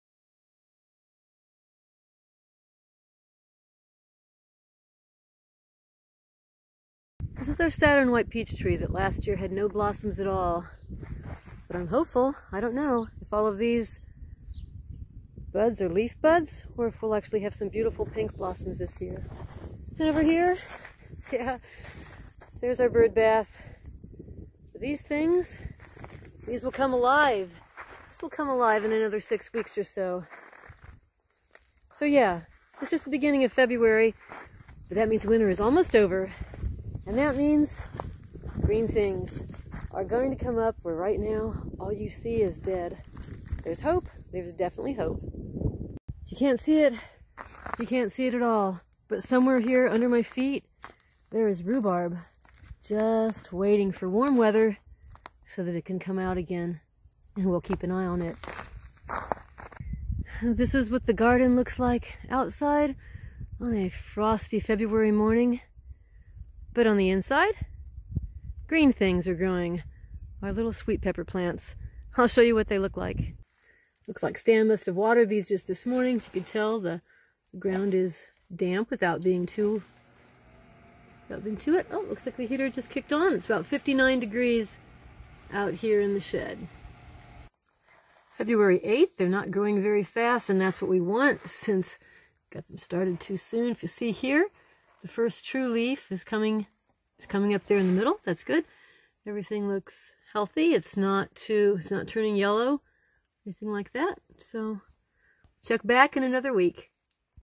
Budding Hopes for Spring!
Hey, I believe this is my 100th video! Lori Fiechter here with a mid-winter garden update. I have been looking for signs of hope, signs of spring. Our garden is dormant, not dead! It is the first week of February...snow on the garden, but green leaves on the pepper plants in the shed.